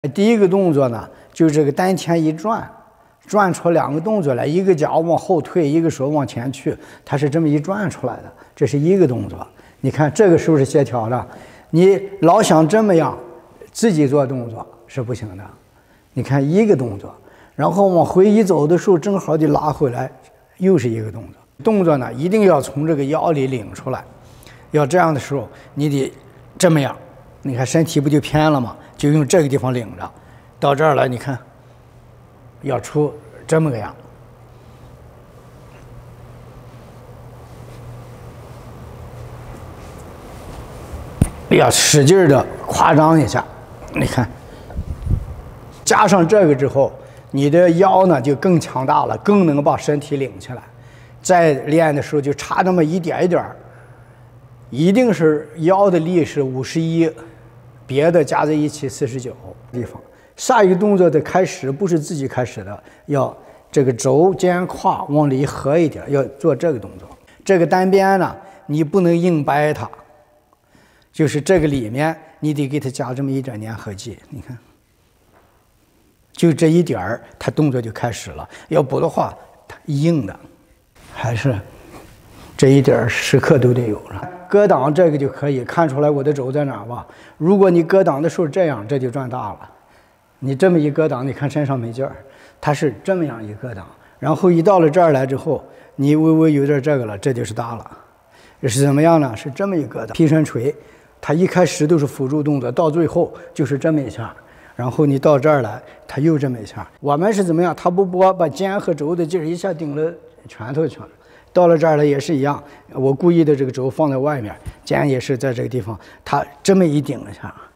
丹田管控花絮-陈中华一路精讲指导 20230116
丹田管控-陈中华一路精讲指导-20230116。用丹田管住所有的动作。否则动作之间会散。全身只有一个劲，一个动作，其余都是结果。有一个动作而引发的很多动作，就像渔网撒出去但都是控制在手里一样，才是一动而无有不动。
用丹田领劲而牵动全身的动，这个时候首先出现的是四肢动的多，出现晃的假象。不要管这些。这是一个练拳的过程不是结果。结果还是要中正。动作不偏不倚，不晃不摆。上下不起伏。
丹田之外再加上肩肘变成肩肘胯领劲，在这里是启动，然后全身动作再开始。这样肩肘胯的内里的动作就起到了粘合剂的作用，是动作不散。 单鞭、六封四闭里都有这个东西。搂膝拗步前两动也是要先做肩肘胯，否则手会动的太多。陈中华# 实用拳法# 丹田管控# 加拿大[地点]#